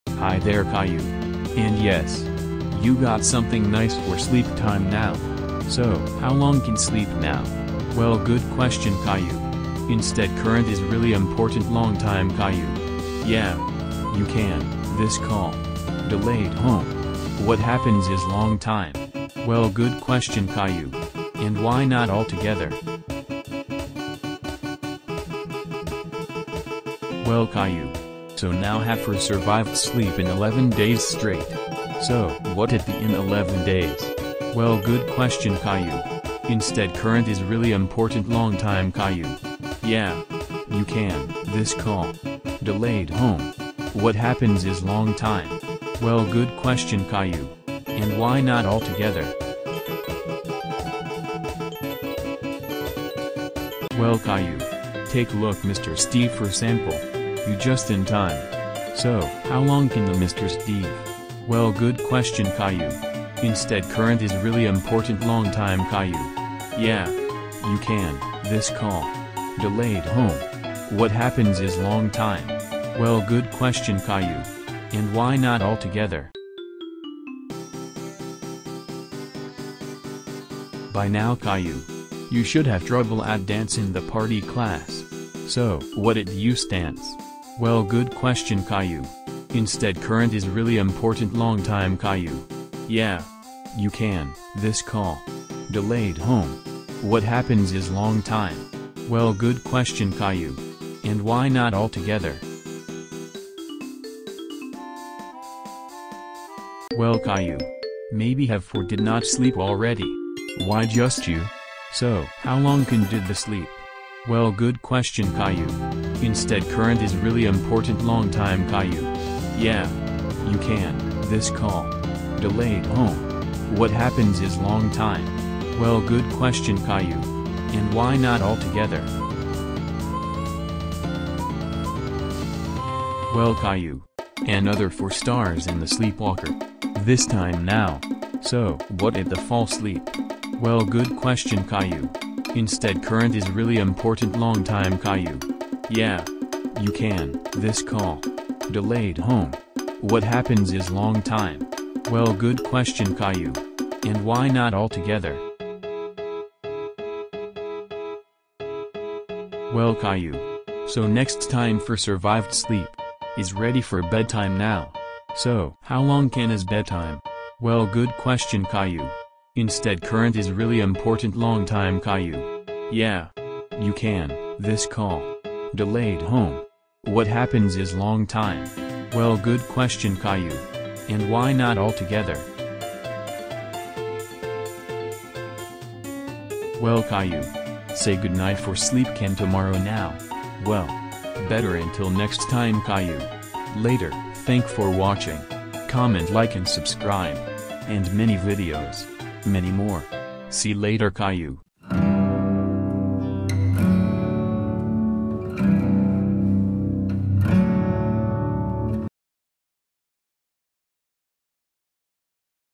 Caillou How Long Can Survive Without Sleep?
is really important long time caillou, then what happens have for sleep, in the 11 days for straight, in fact caillou, take look for mr steve sample, you just for 11 days, and when you for die, this caillou, and bedtime very happy